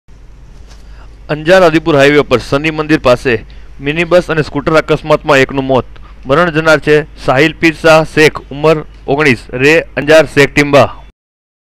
SAJAG KUTCH _27_FEB_2012.mpg
અંજાર આદિપુર હાઈવે પર આવેલ સની મંદિર પાસે લક્ઝરી અને સ્કુટર અકસ્માતમાં અંજાર ના શેખ પીર વિસ્તાર માં રહેતા આશાસ્પદ યુવાન શાહીલાપીર શા શેખ નું અવસાન .